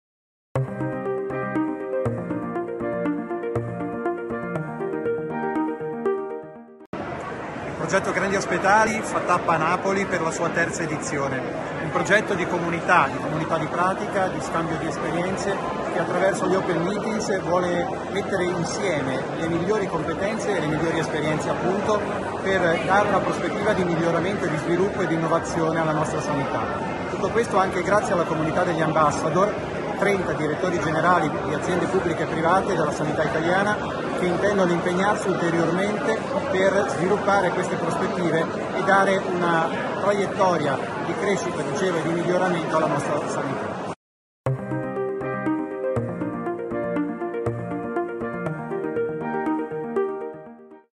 Paolo Petralia (vicepresidente Fiaso): Grandi Ospedali progetto per migliorare la nostra sanità
Paolo Petralia, vicepresidente vicario di FIASO, ha dichiarato: “Grandi Ospedali è un progetto di comunità che mira a migliorare e innovare la nostra sanità. Grazie agli Open Meetings, mettiamo insieme le migliori competenze e esperienze per sviluppare nuove prospettive di crescita"